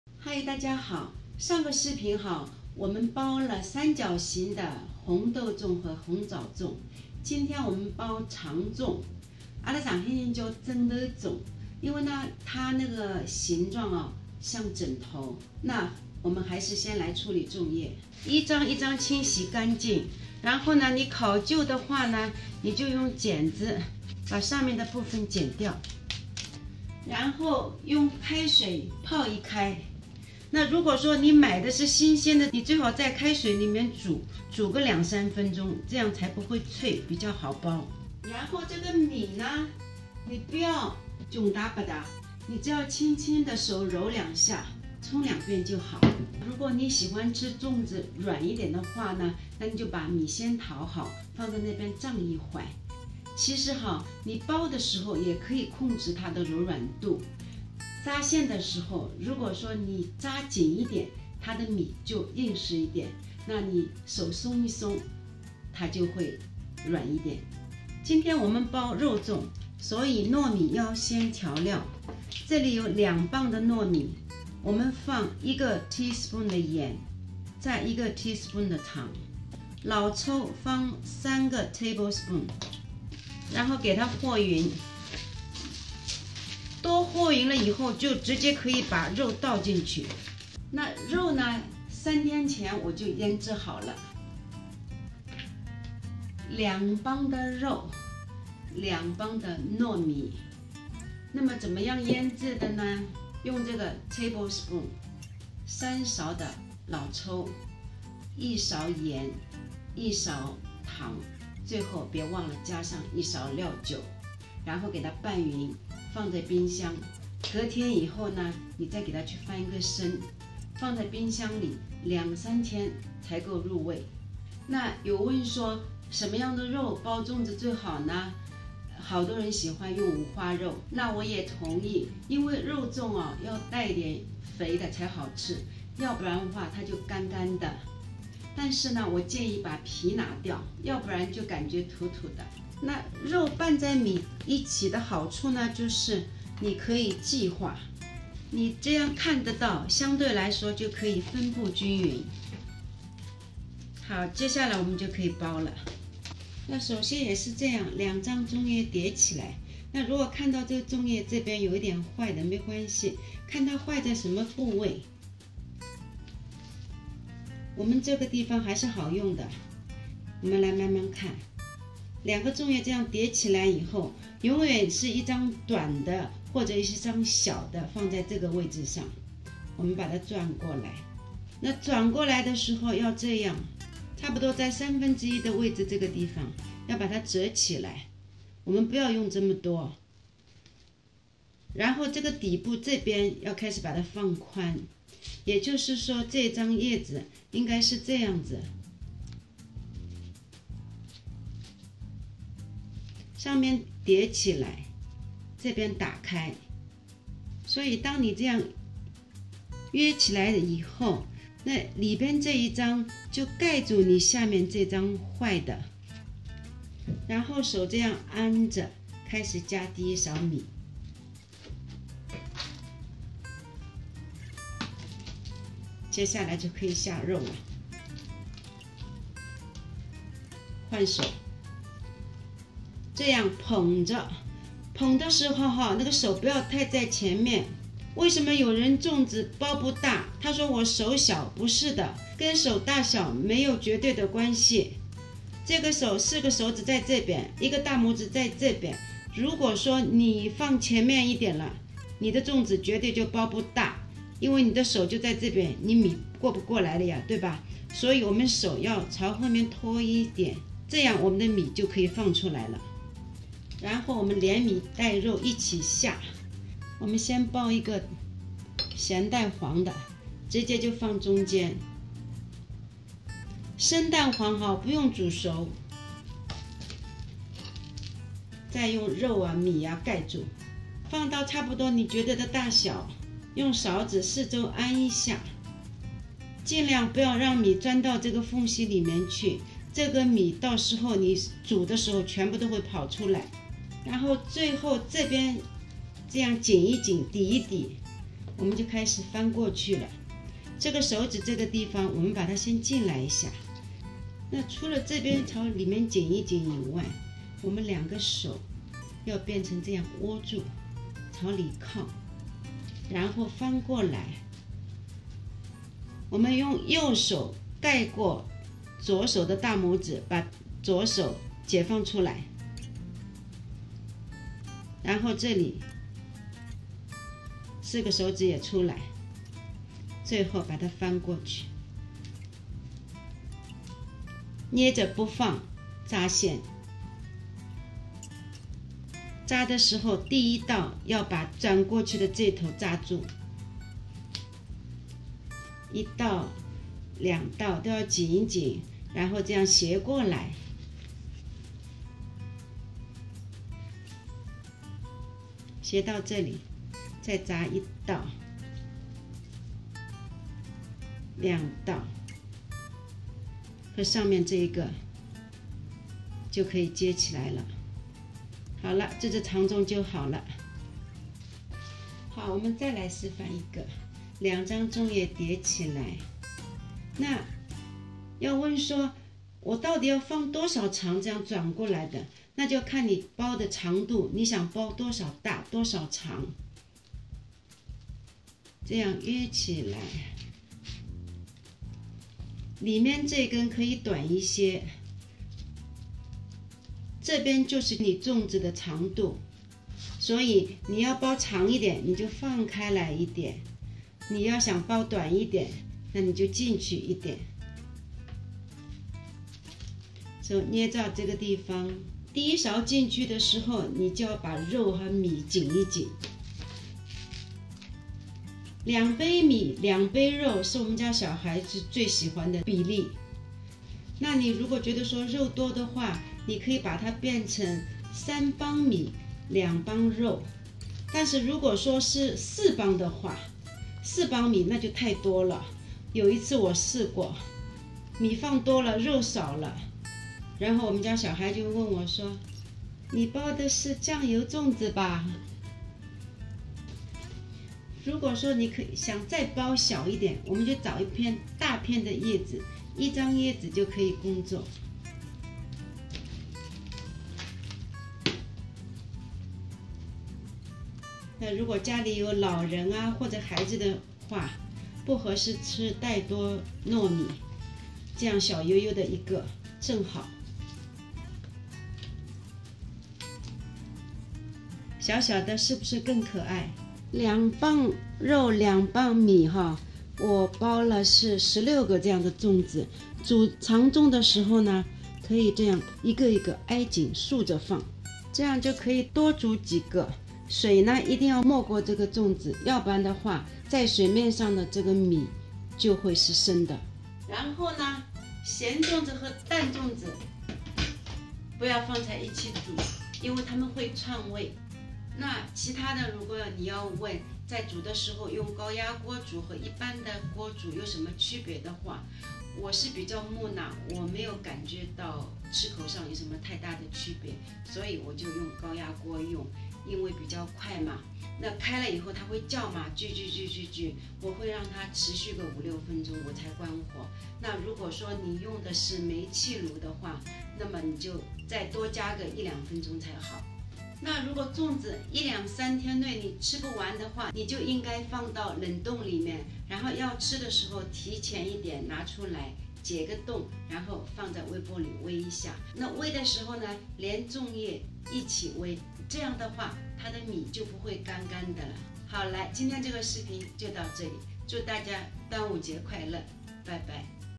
包粽(二)醬肉蛋黃長粽又名枕頭粽！
Recipe:

Pork Belly (Skinless) - 2 lb
Dark Soy Sauce - 3 Tbsp
Cooking Wine - 1 Tbsp

Marinade the pork with the soy sauce, sugar, cooking wine and salt for a minimum of 3 days